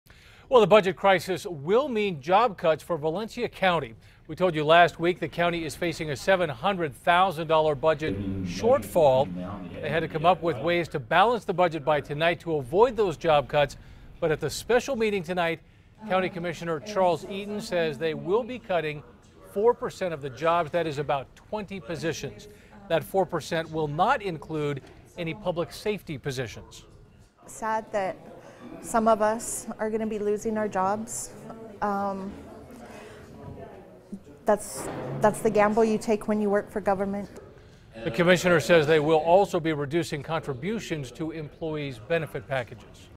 Valencia County decides to cut jobs due to budget shortfall
Valencia County decides to cut jobs due to budget shortfall - Source